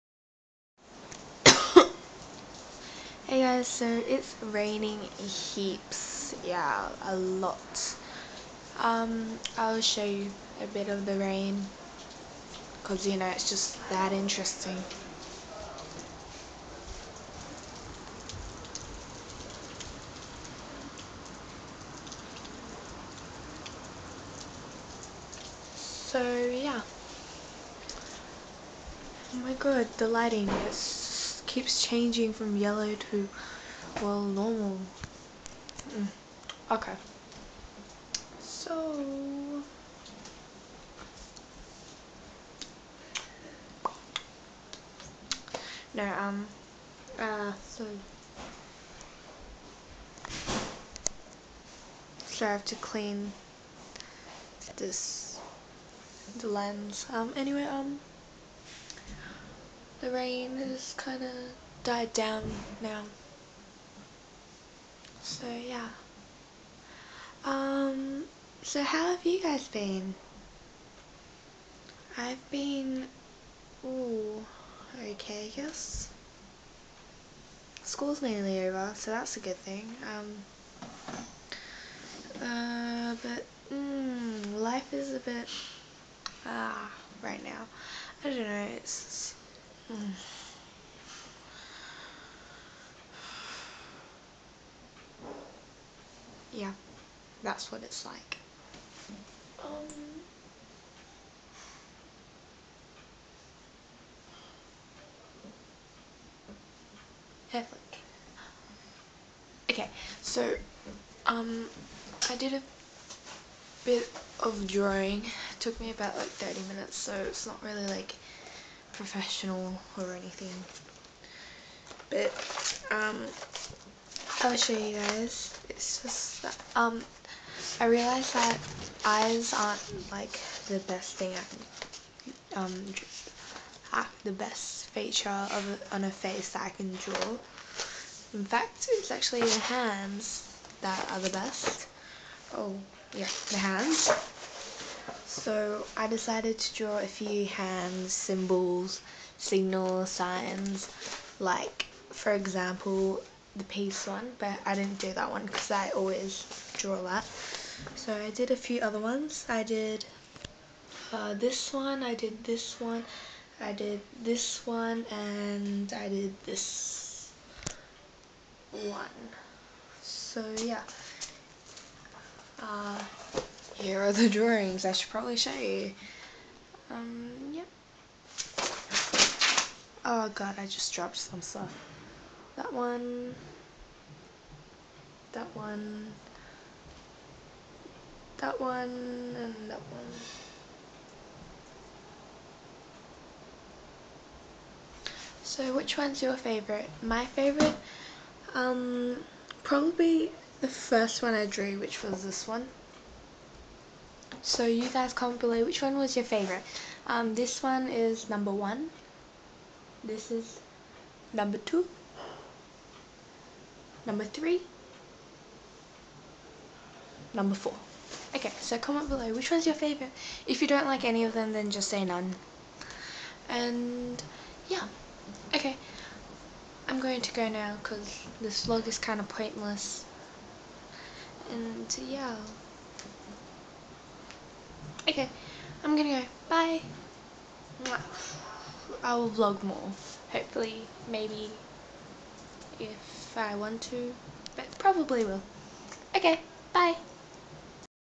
Drawings of hands *Vlog 15*
Fun fun fun. I like drawings hands c: So which drawing was your favourite?

I would have put this on my main channel as a question with boxhead vid but I thought that it was a bit more vloggy and I haven't posted much videos here so yeh :)